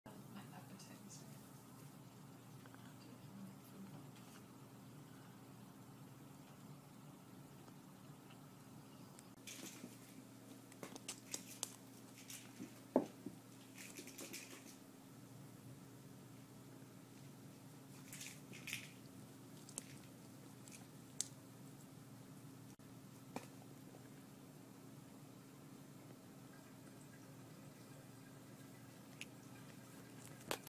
Kittens and Play Cube
Amelia is teething and sometimes nibbles on the cube edges.